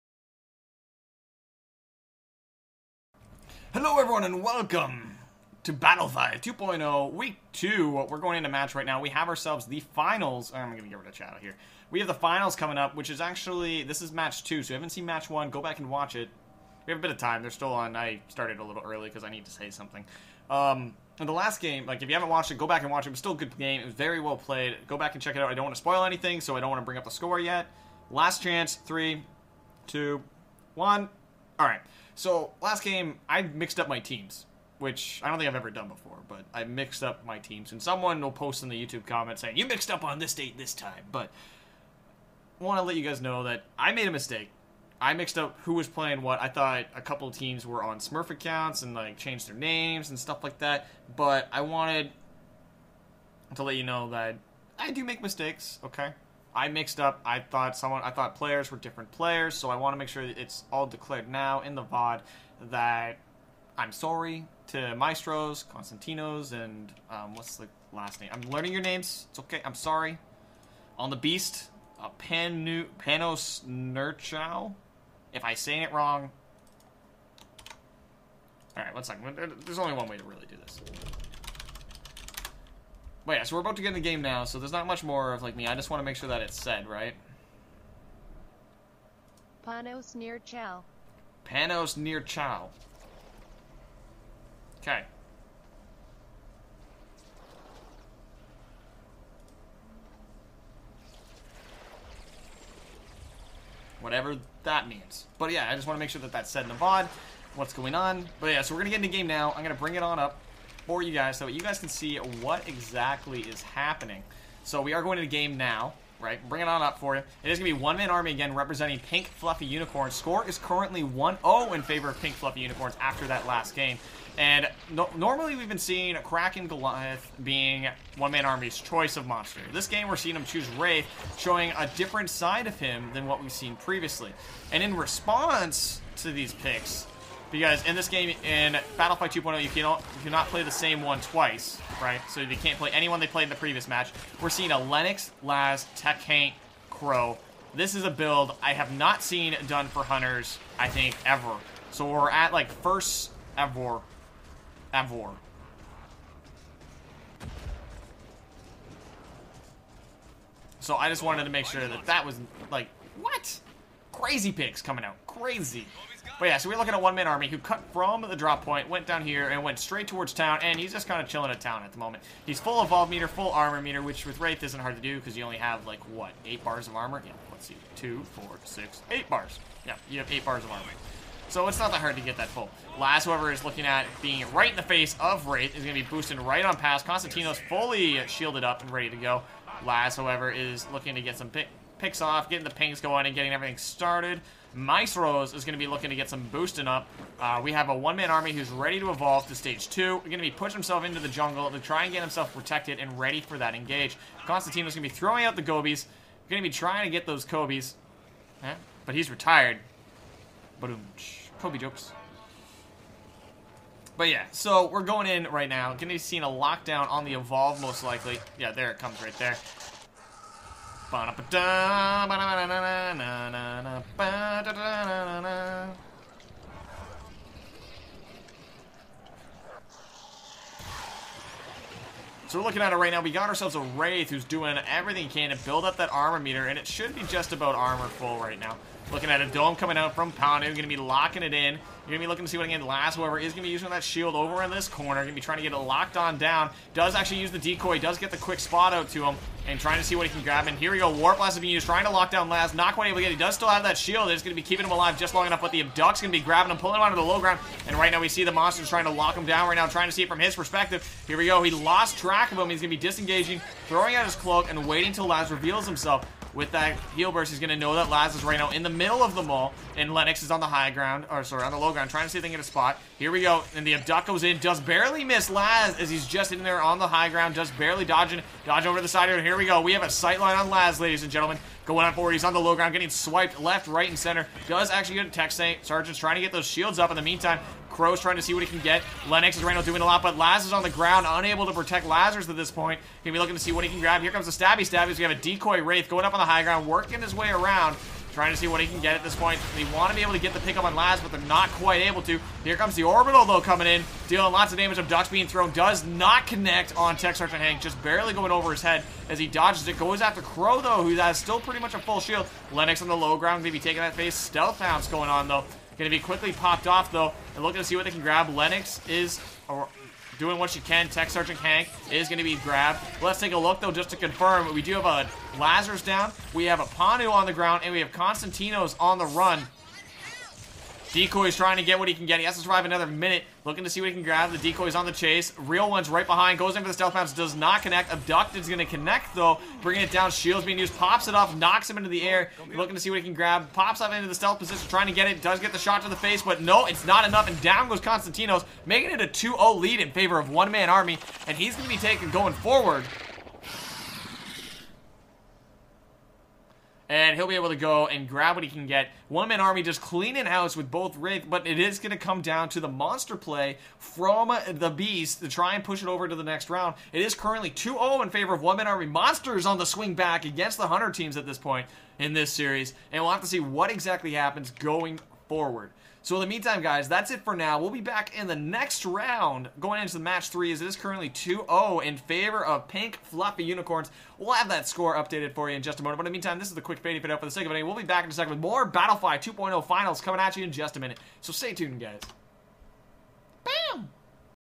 BattleFY 2.0 Pink Fluffy Unicorns vs The Beast Match 2
BattleFY 2.0 Community Cup is a Bi-weekly tournament where teams compete for skins!
Matches are played Simultaneous up until the tiebreaker.

During the recording of this match, I made a mistake and mixed up a group of players. This was addressed in the second video of the series. Again, i apologise to the players affected and will not make that mistake again, in the future. Sorry guys/gals :(

We will be broadcasting as many of these games as possible, so long as time permits.